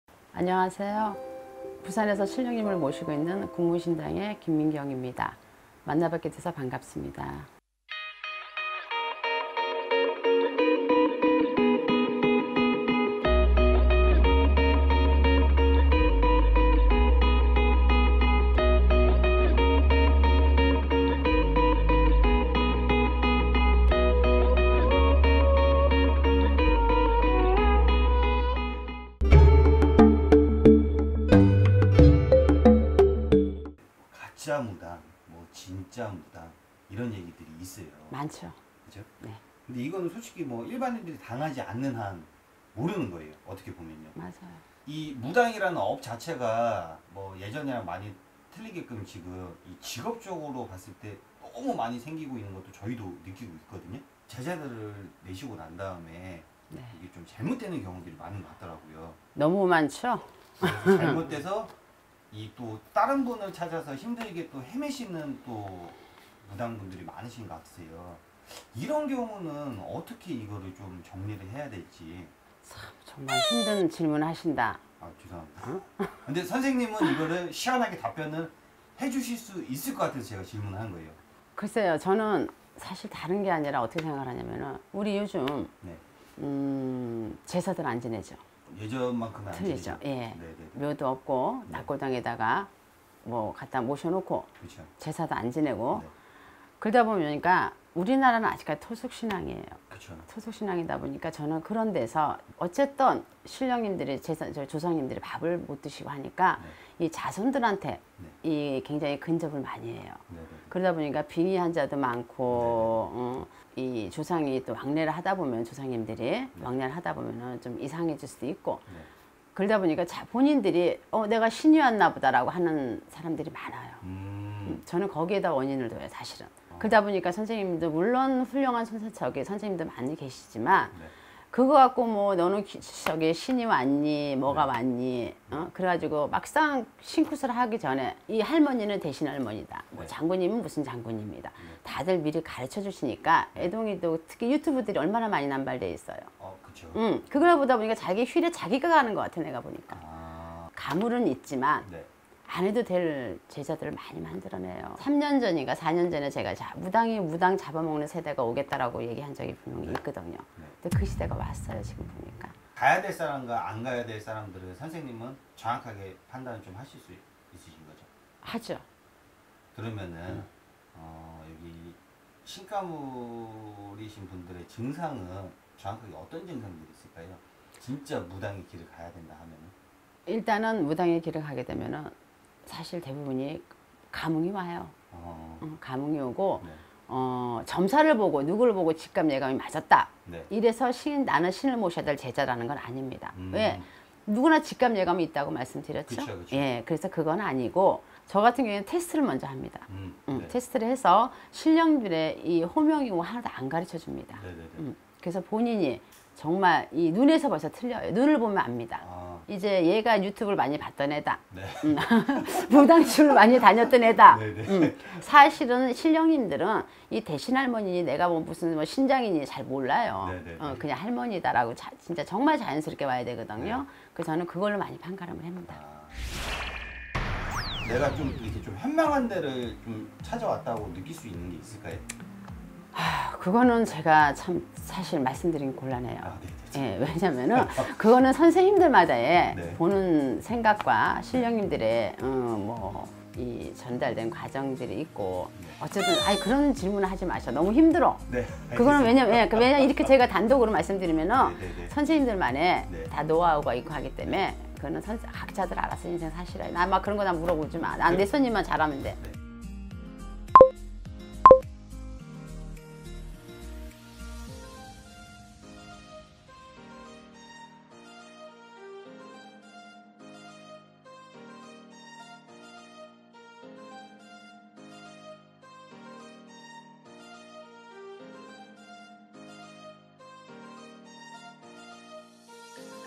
★신을 모셔야 하는 고민 해결!★ 진짜무당이 말하는 가짜무당이 판을 치는 세상 부산점집 부산 국무신당
자막

★대표블로그★
-올마이티TV